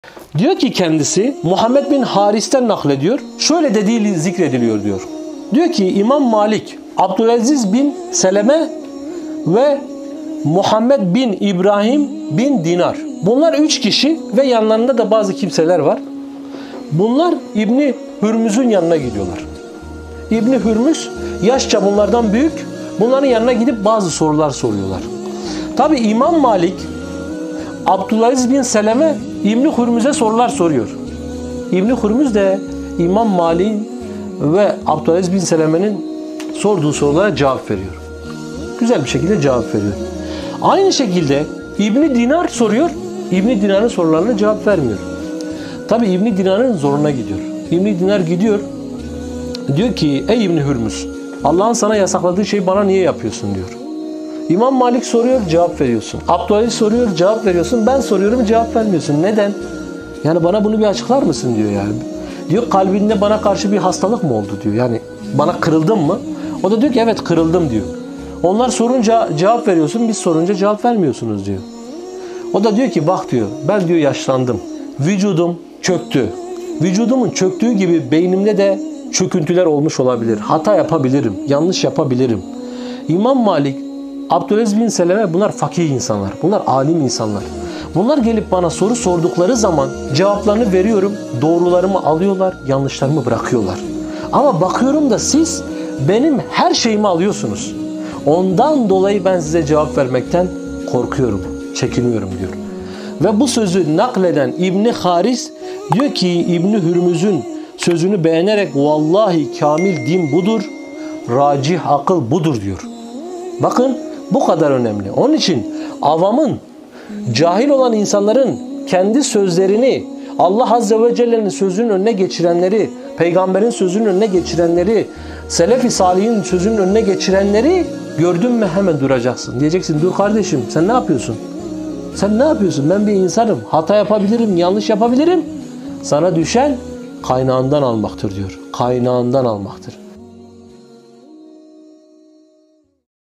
Alimlerin Her Sözünü Almak Kötülük Olarak Yeter / Ebu Nuh Hoca
Ebu Abdullah devamla şöyle diyor: Muhammed b. Haris, Sahnun b. Said'in naklettiği haberler arasında onun şöyle dediğini zikrediyor:

İmam Malik, Abdulaziz b. Seleme, Muhammed b. İbrahim b. Dinar ve benzeri diğer arkadaşları İbni Hürmüz' ün yanına ilim için gidip geliyorlardı.
İbni Hürmüz'e imam Malik ve Abdulaziz bir şey sorduklannda cevap veriyor, îbni Dinar ve benzerleri
bir şey sorduğunda onlara pek cevap vermiyordu. Bir gün İbni Dinar, îbni Hürmüz'e itiraz ederek, Ya Eba Bekir! Sana helal olmayan bir şeyi bana karşı nasıl helal sayıyorsun?Malik ve Abdulaziz bir şey sorduklarında cevap veriyorsun, ben ve arkadaşlarım bir şey sorduğumuzda bize cevap
vermiyorsun' dedi.
İbni Hur-muz: 'Ey kardeşimin oğlu! Bu durum kalbinde bir şey mi meydana getirdi ?' dedi. İbni Dinar: 'Evet' dedi. İbni Hürmüz: 'Benim yaşım büyüdü, kemiklerim zayıfladı.
Ben cismimi böyle zayıflatanın aklımı da zayıflatmış olmasından korkuyorum. Malik ve Abdulaziz alim ve fakih kimselerdir, benden doğru bir şey işittiklerinde kabul ediyorlar, fakat doğru olamayan yanlış bir şey işittiklerinde onu terk ediyorlar. Sen ve arkadaşların cevap verdiğim her
şeyi kabul ediyorsunuz' dedi.
İbni'l-Haris: (İbni Hürmüz'ün sözünü beğenerek) 'Vallahi kamil din ve racih akıl budur. Böyle bir kimse kendi indinden getirdiği hezeyan ve görüşlerini insanların gönlünde Kur'anın yerine koymak isteyen kimse gibi olacak değil ya'diyorlar.